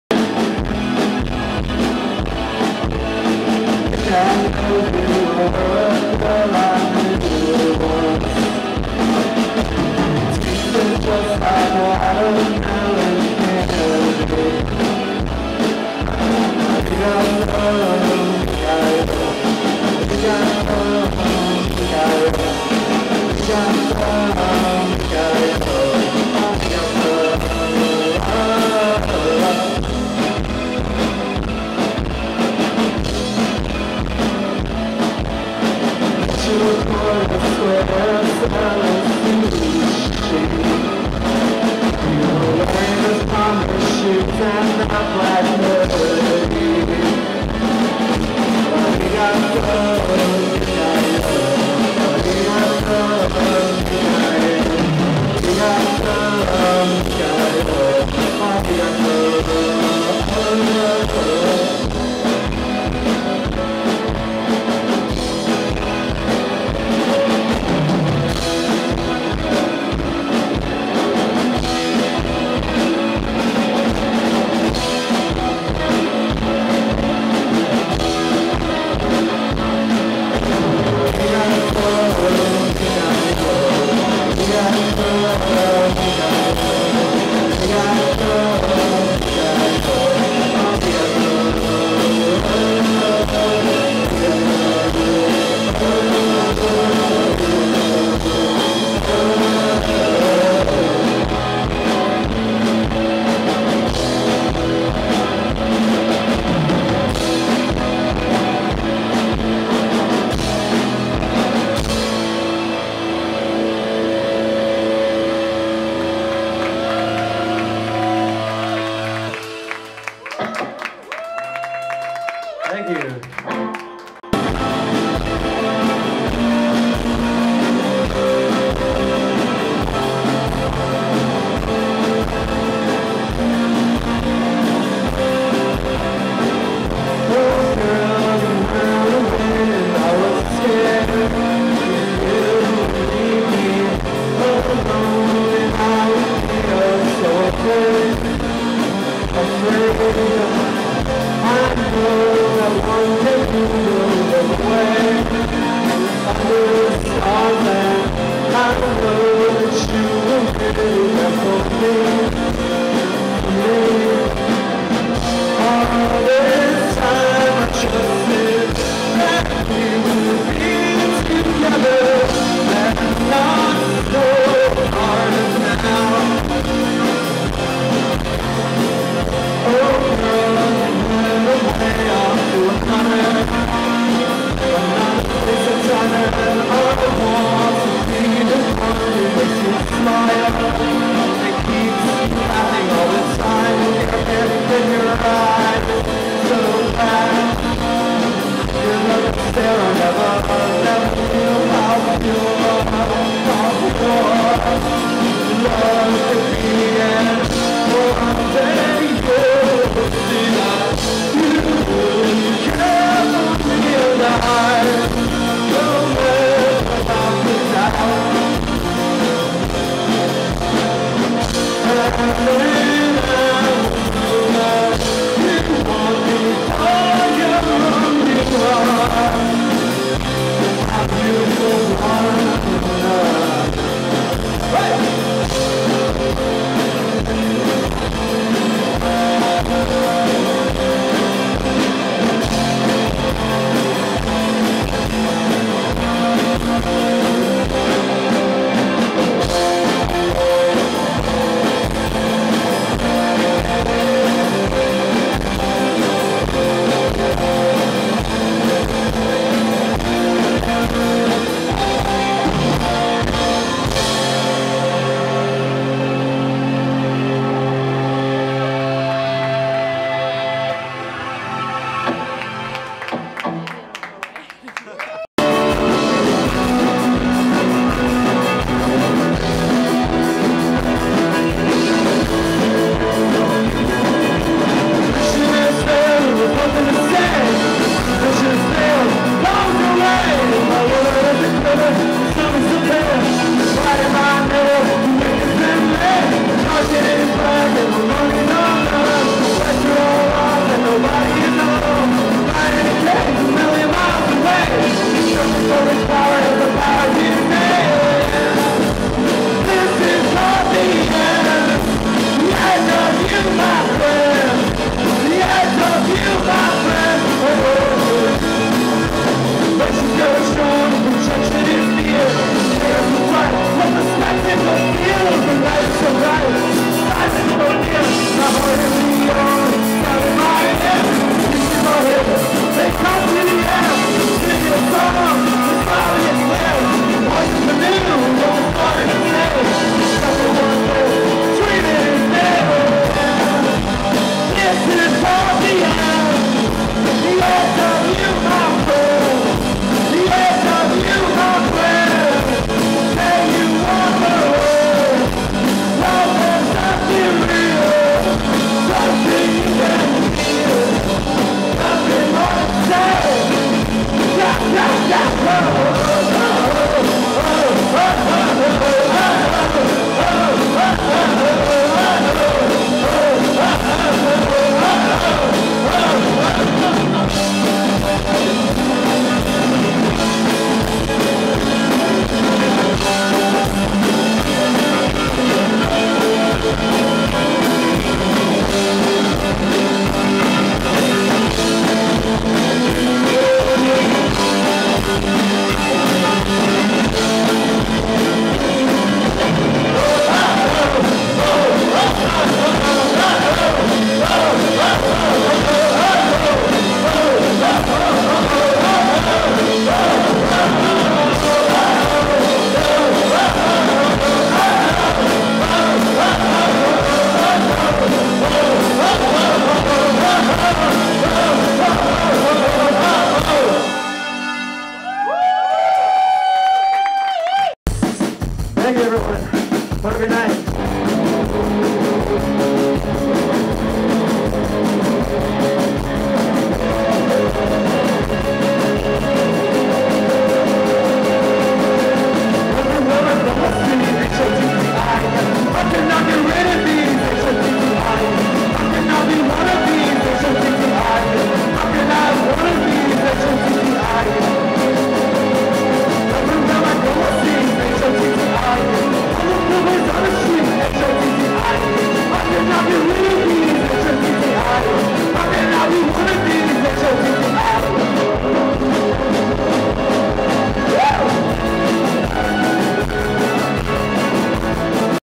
Streight Angular @ Great Scott (2.03.11)
The band plays in compact form at Great Scott in Allston. They play a
few new songs, too!